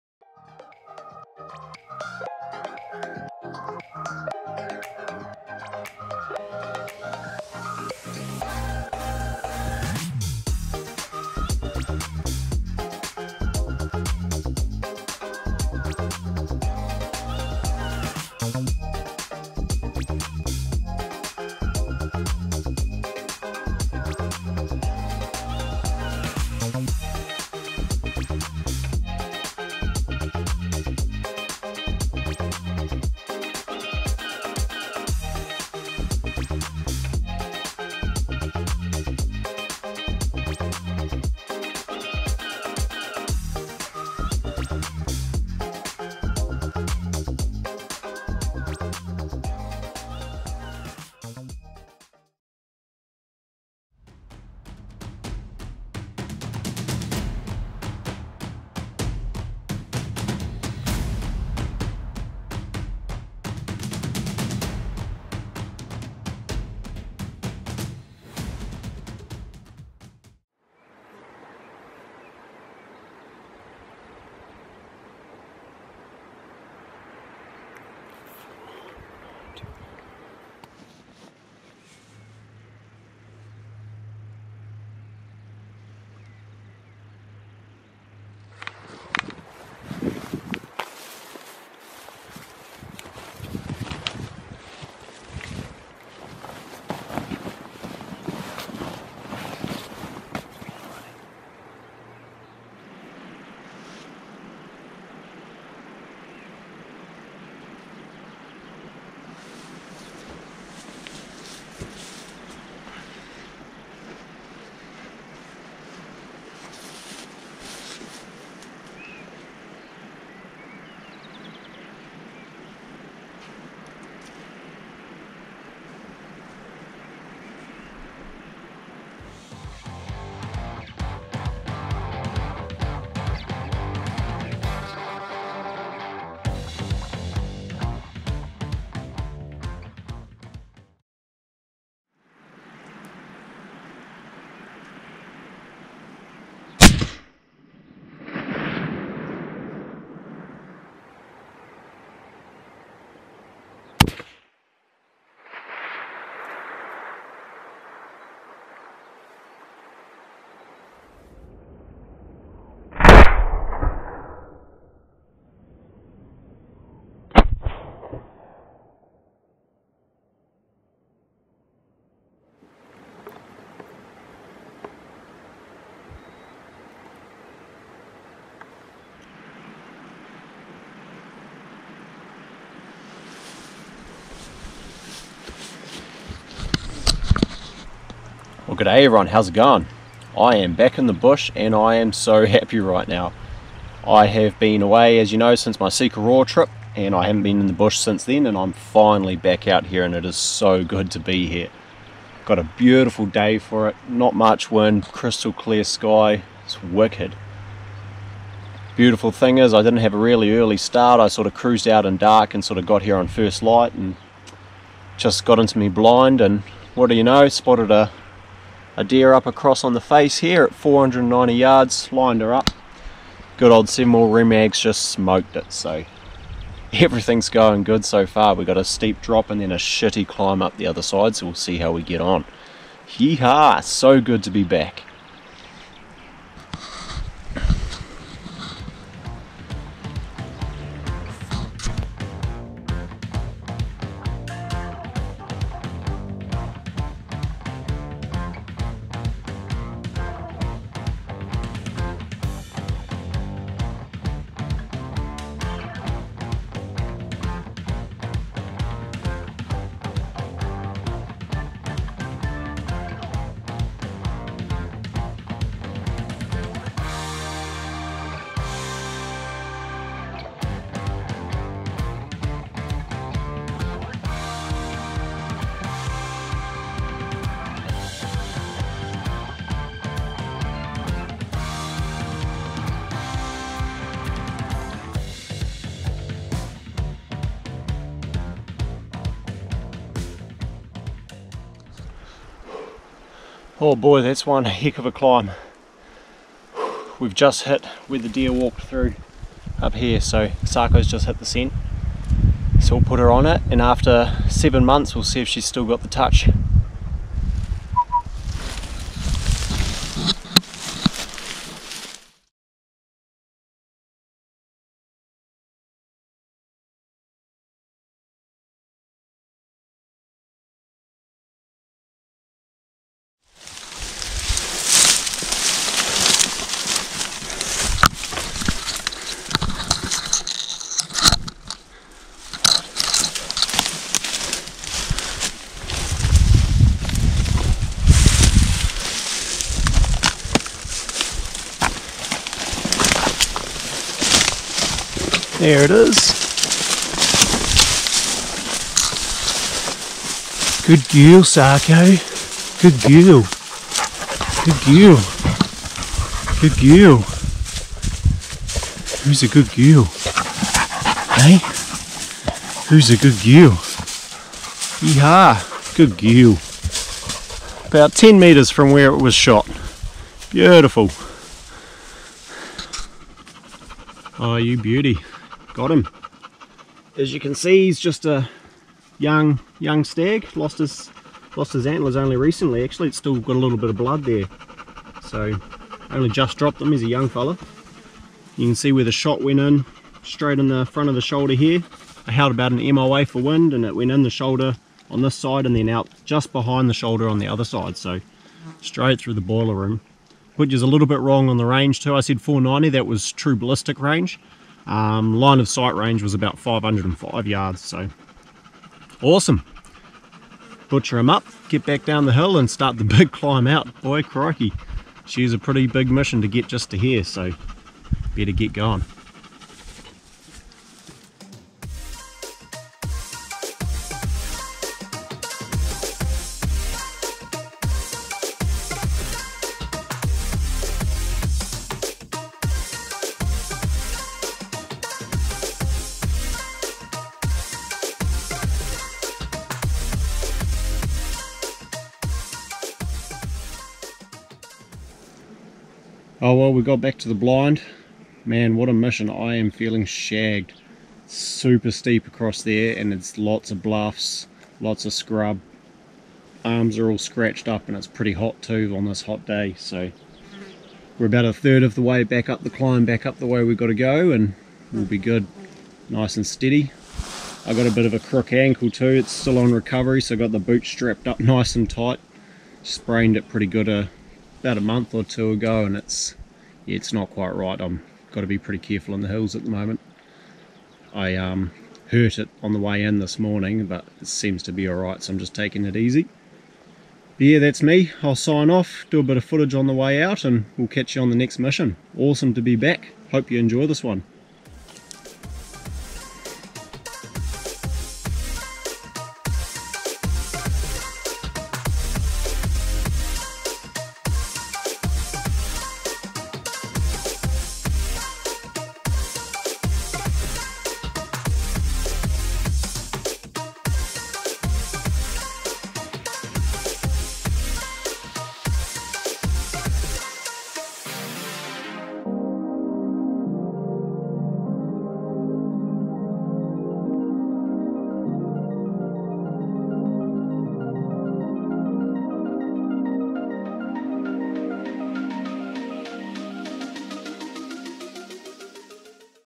Spring Red Deer Success
First mission back in the hills since the roar results in an awesome big day on the hill and some venison for the freezer.